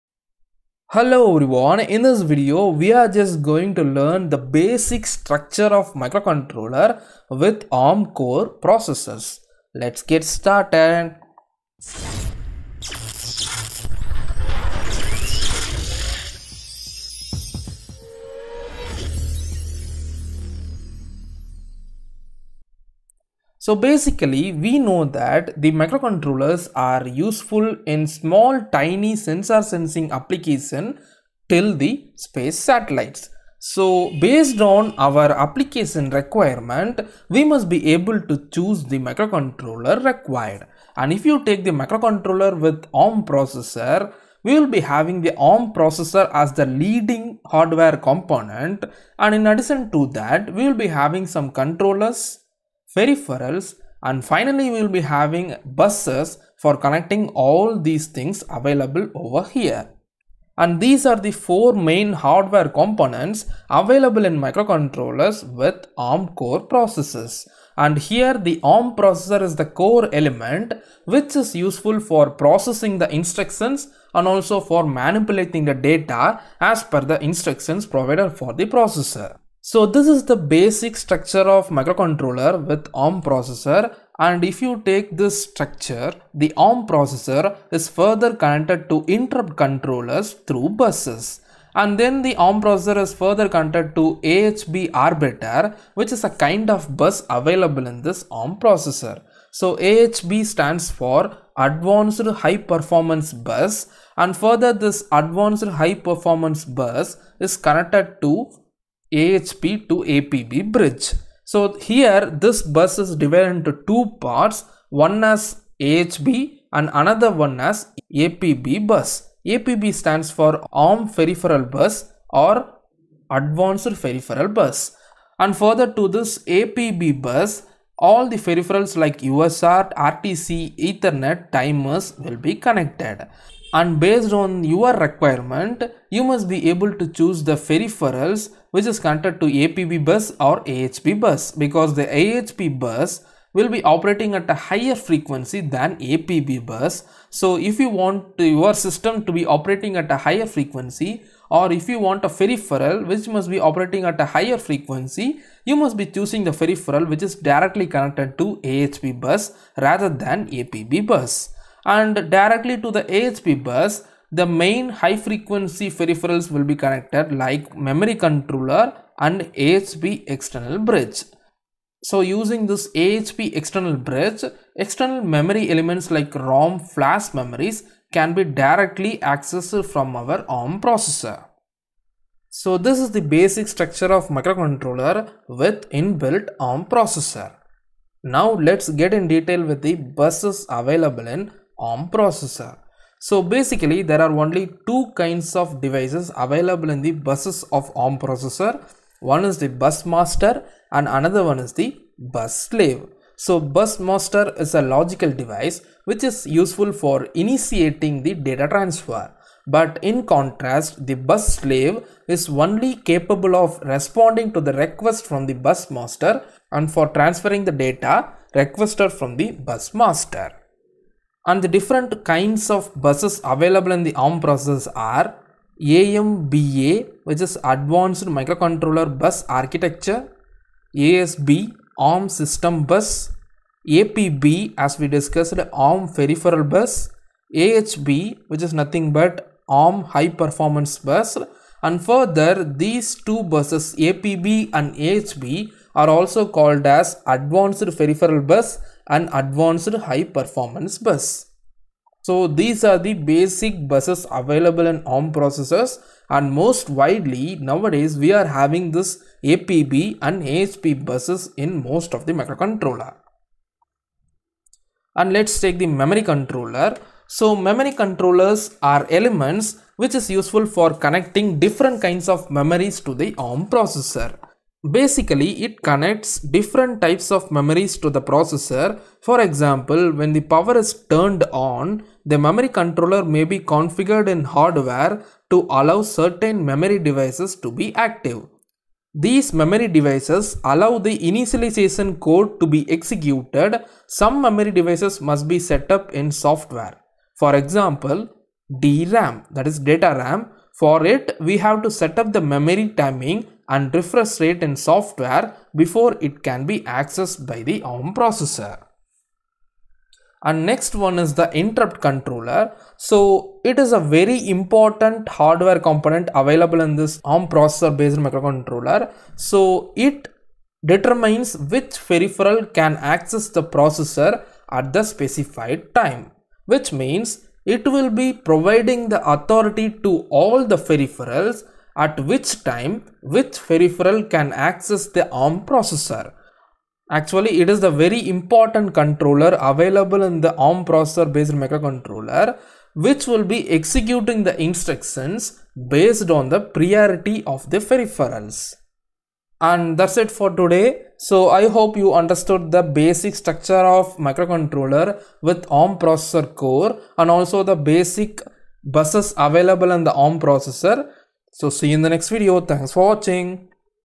ARM Buses & Its types | Embedded Systems with ARM Processor Explained
This video will get to some knowledge on Bus Architecture of ARM Processors and Microcontroller with ARM processors, This is a course on Bare Metal programming in KEIL IDE  for the TM4C123GXL microcontroller , For learning this course for free with all the documents and necessary source code for all the lectures

Here is the playlist for Introduction to Bare Metal Programming Course in ARM Cortex-M (TM4C123GXL):

Downloads:
Log into the course for free and you can access all the resources as necessary